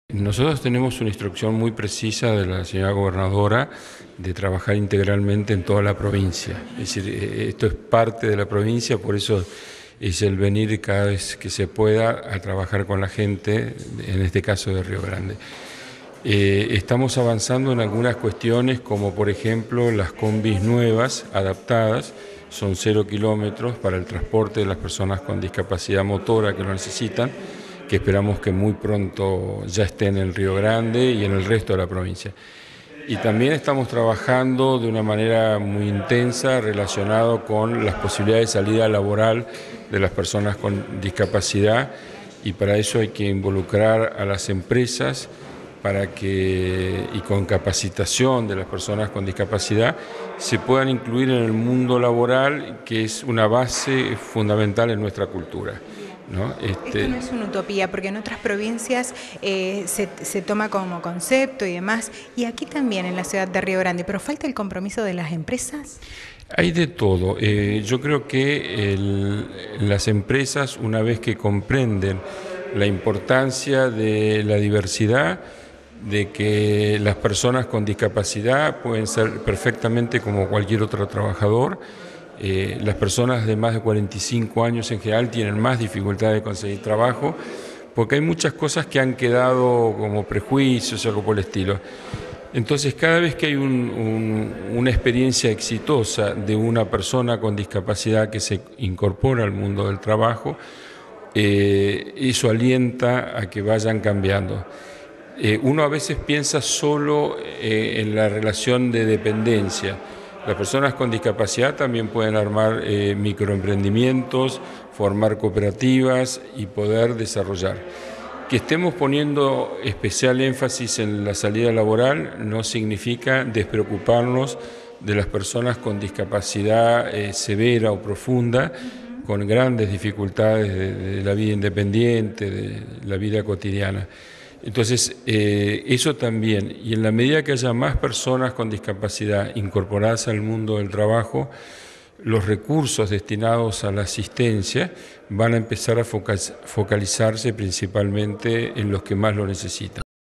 Trabajo integral en toda la provincia
Dialogamos con el Secretario de Discapacidad Provincial, Alejandro Rojo Vivot, en relación a las gestiones para la inclusión laboral que se realizan con las empresas fueguinas.